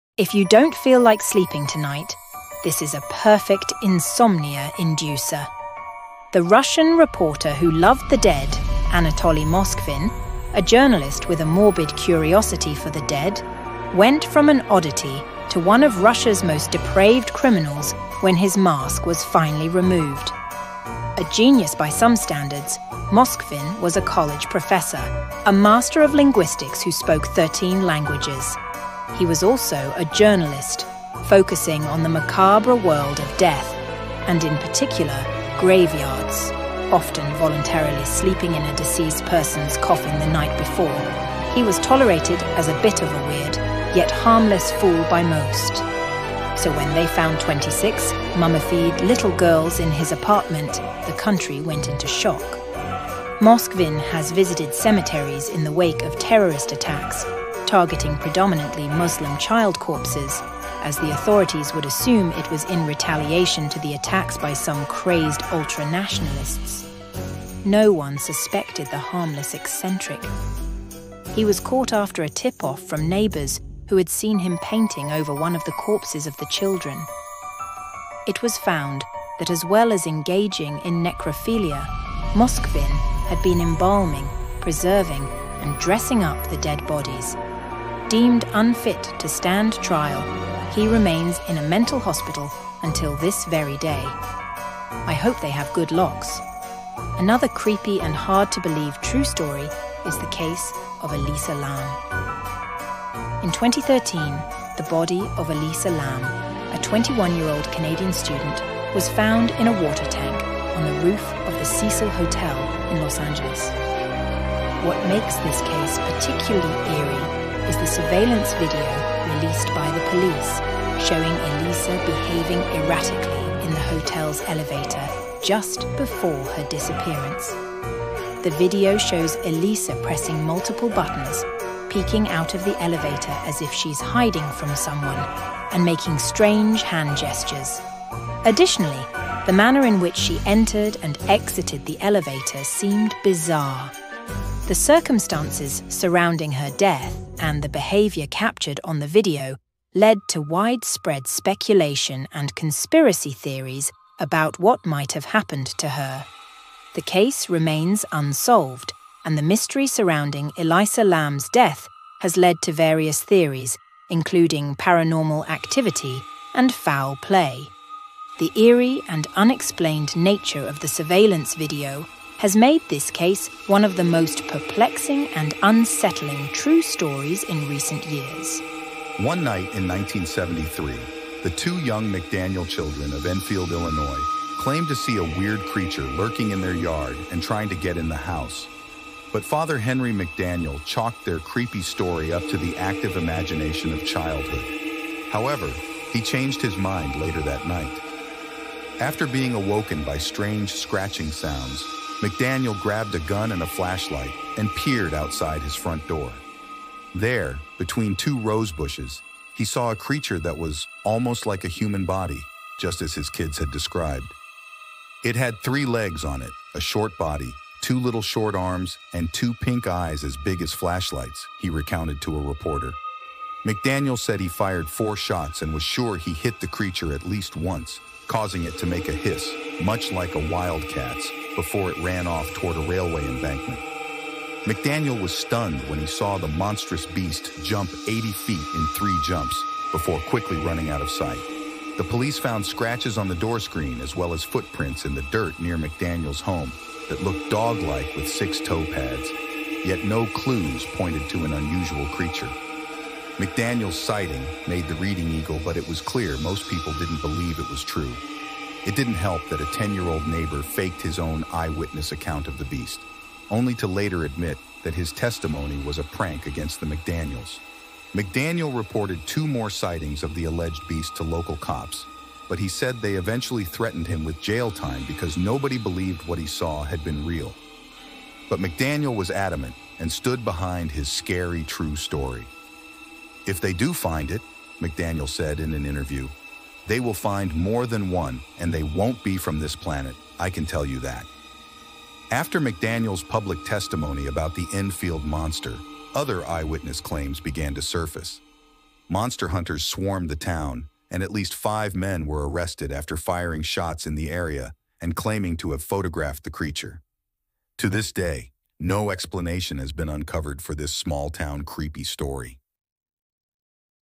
True SCARY Stories that are EVIL!
This is 3 Scary True Stories, inspired by the countless top 5 scary stories or even top 10 or top 20 horror stories.

Anatoly Moskvin is on of the central subjects, a true crime folklore legend, as well as the Enfield monster.